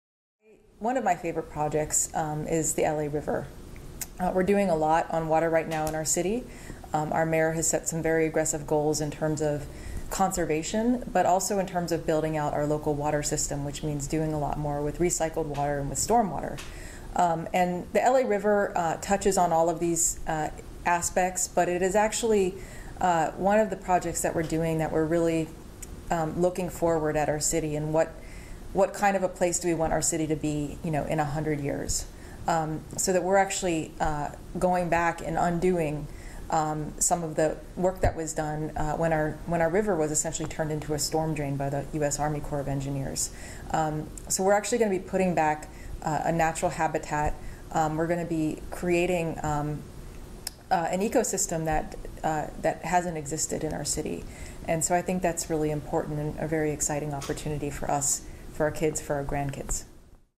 ‪#‎InfrastructureMatters‬ to the ‪#‎LARiver‬
‪#‎InfrastructureMatters‬ to the ‪#‎LARiver‬, explained by BPW Commissioner Heather Repenning. ‪#‎PublicWorksWeek‬